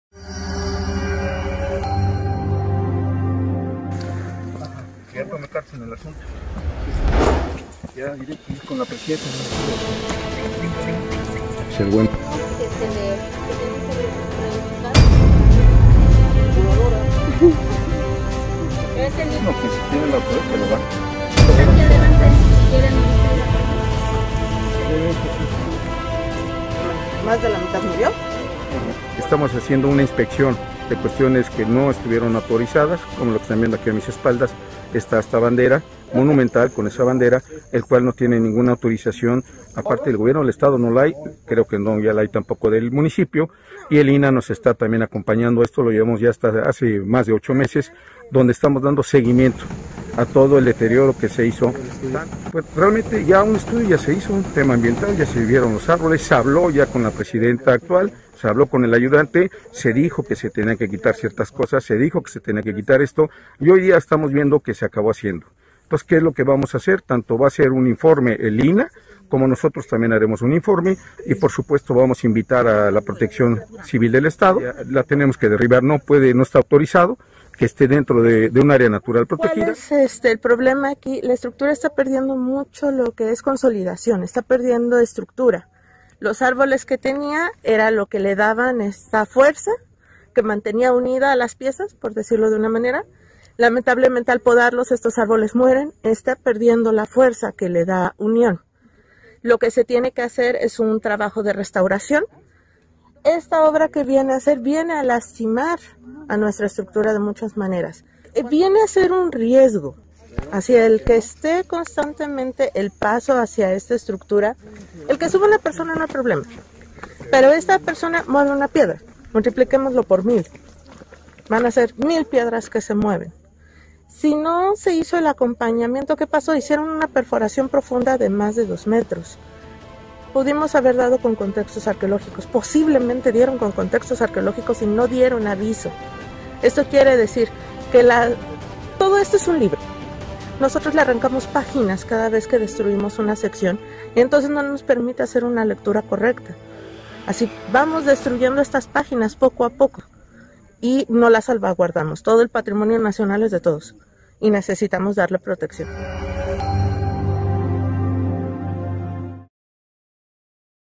DAÑARON ÁREA NATURAL PROTEGIDA EN ZACATEPEC
CARECE DE TODO PERMISO EL ASTA BANDERA CONSTRUÍDA EN EL CERRO DE LA TORTUGA
• Daña esta Área Natural Protegida por lo que se requerirá su remoción
• Pudieron haberse encontrado vestigios arqueológicos y no se dio aviso al INAH